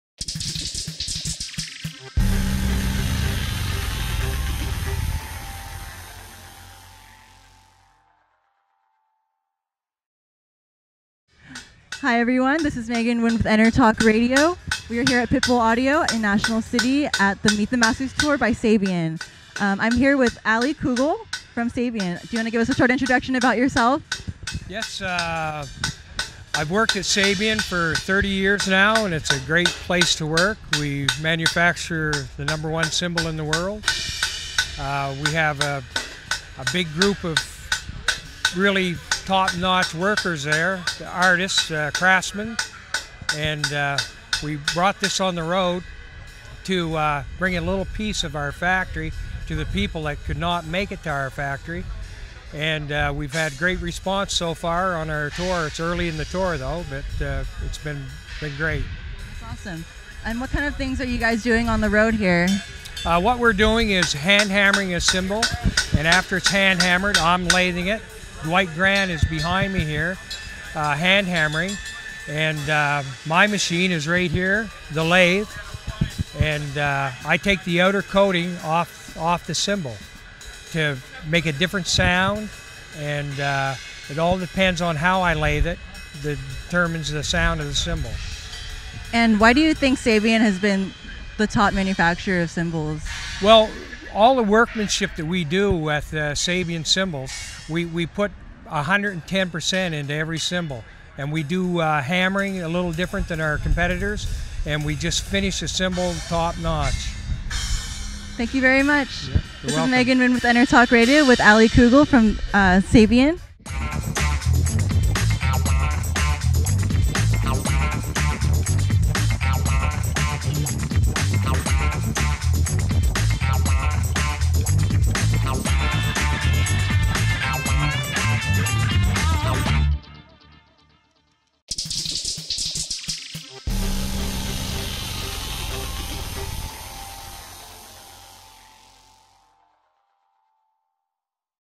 Sabian Meet the Masters Tour: Allie Cougle
The Sabian Meet the Masters Tour came down to our store in National City and literally brought the factory to us!  Some of their most experienced and talented cymbal makers came down to show us the process!  EnterTalk Radio Correspondent, Megan Nguyen talks with Allie Cougle, who is showing us the lathing technique and how Sabian has been the top cymbal company for so long!